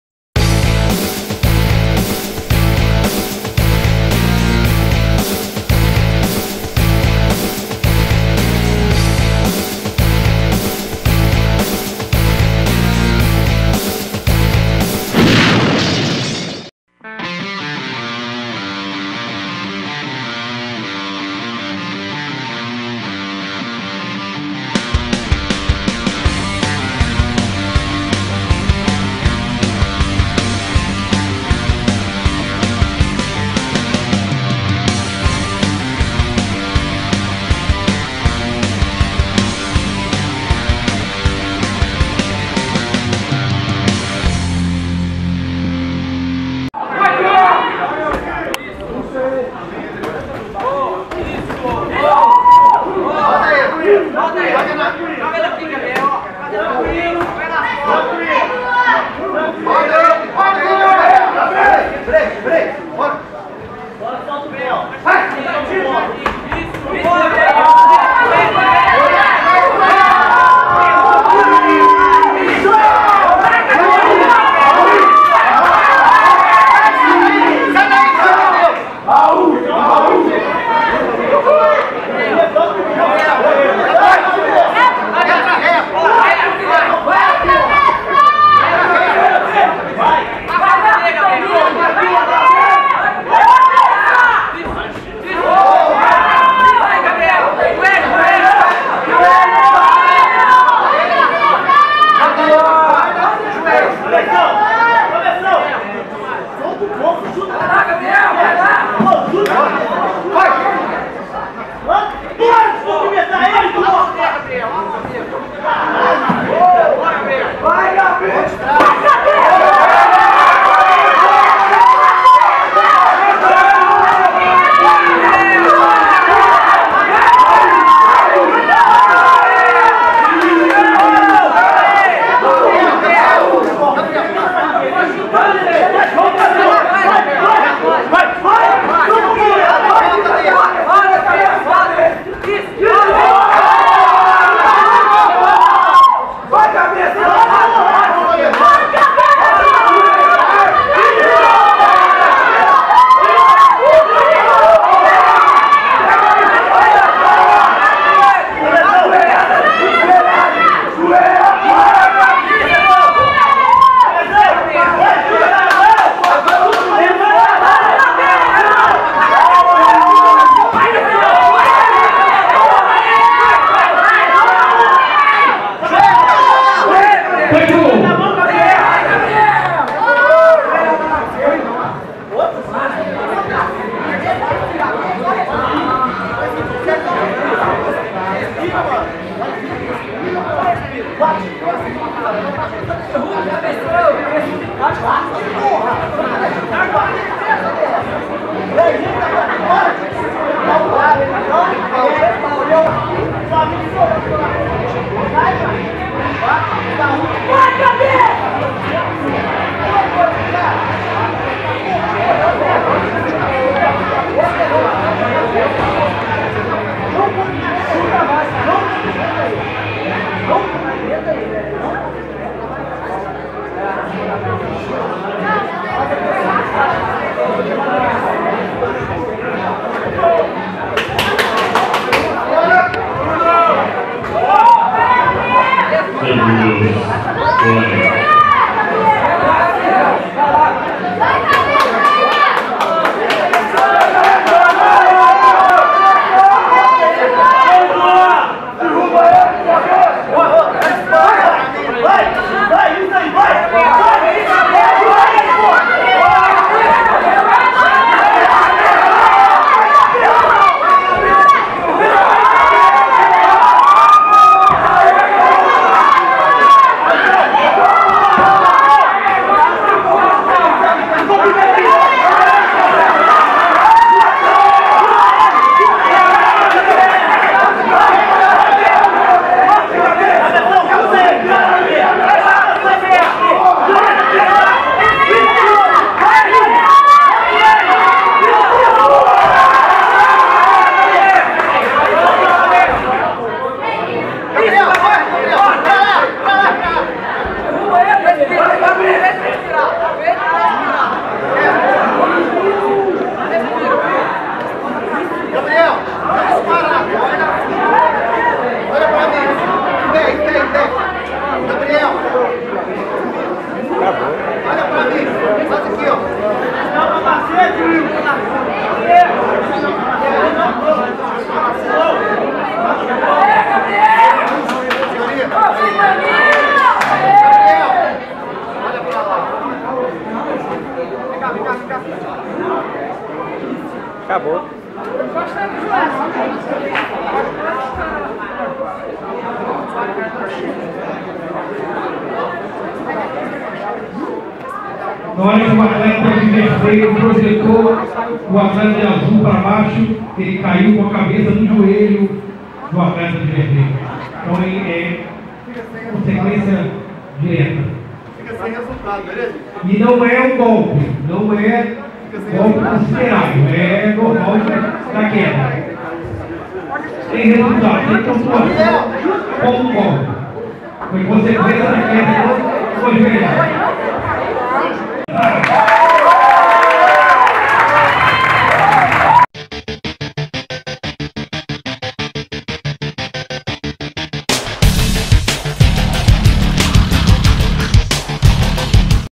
Gabriel vs Eduardo - Campeonato Mineiro 2014 CBKT
Gabriel Cesar (Combat Clube) venceu Eduardo Pereira (Fagner Rakchal) por decisão médica.